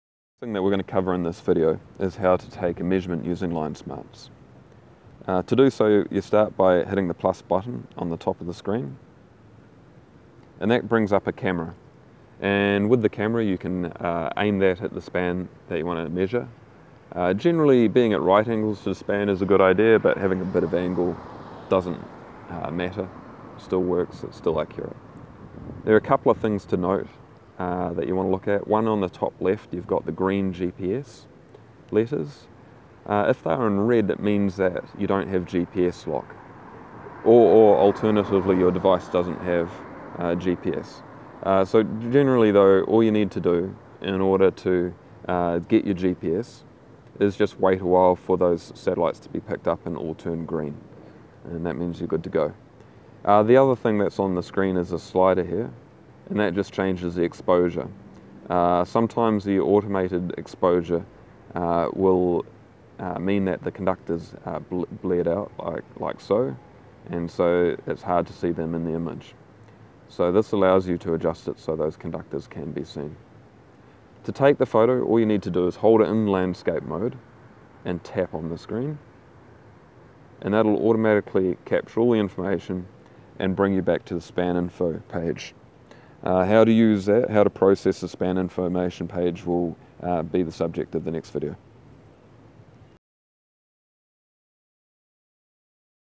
LineSmarts - Measurement capture (V31)
This video shows how to capture a measurement using LineSmarts